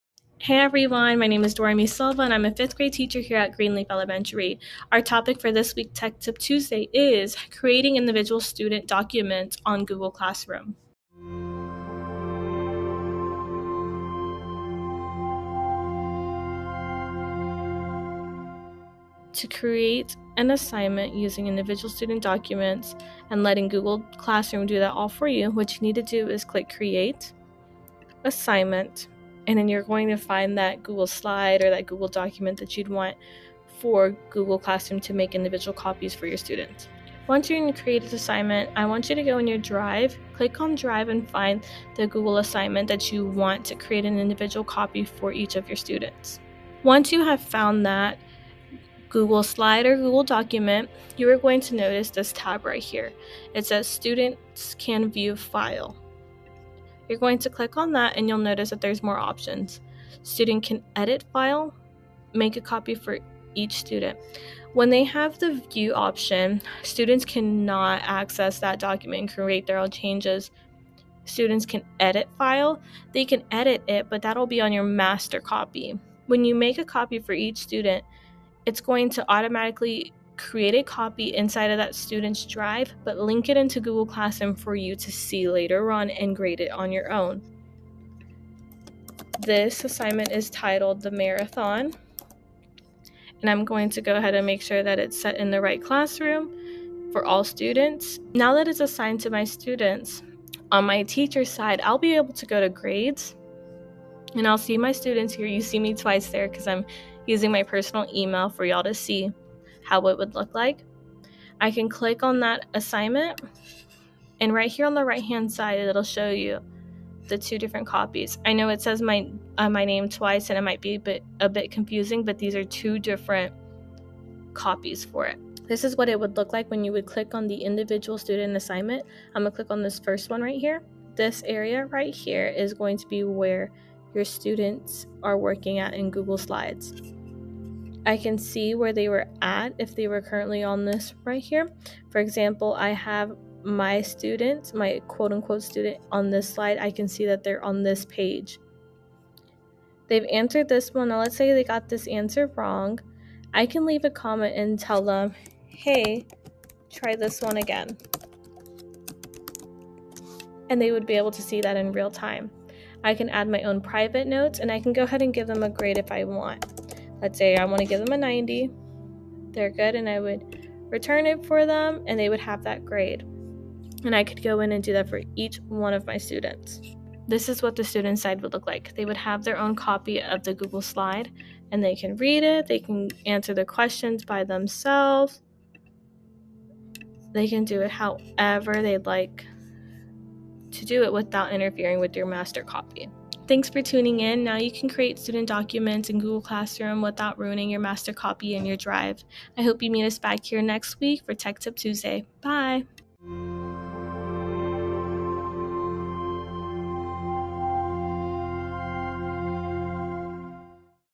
Tech Tip Tuesday S05E20 Individual Student Documents in Google Classroom
This week, we will learn how to guide students in creating their own copies of documents that have been shared with them in Google Classroom.